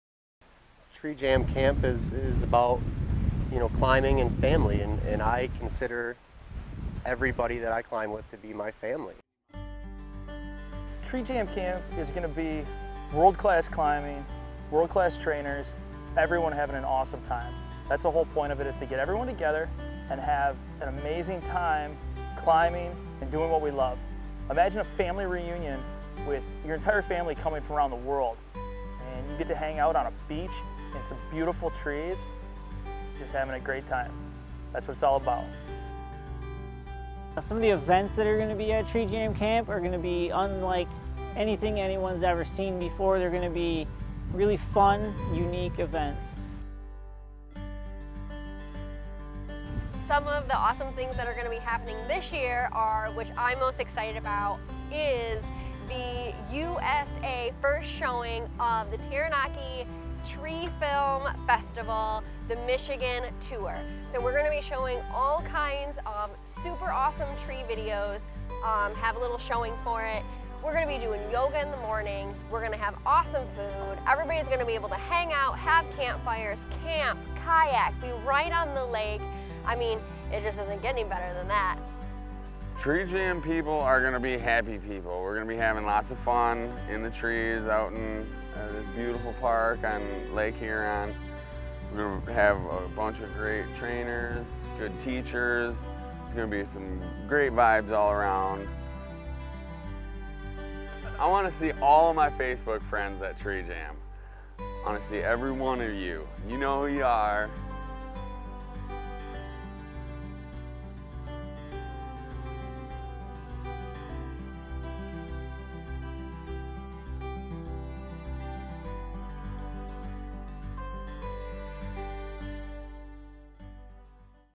TREE JAM CAMP - What it's all about!
TREE-JAM-CAMP is a family reunion for tree climbers of all ages and abilities. We want everyone in the arborist industry to get together and share all the knowledge we have gained. We want your families to get together to inspire younger generations to climb trees and appreciate nature. Bartlett Arborist Supply is proud to bring this to the entire tree climbing industry!
Thank you to Bartlett Arborist Supply, Aaron Johns, Joseph Smith, Rich Ellison, Autumn Carufel, Kevin Bingham ( and family), Mike Zettle, Joe Drinkhorn, Migrove, and Vienna.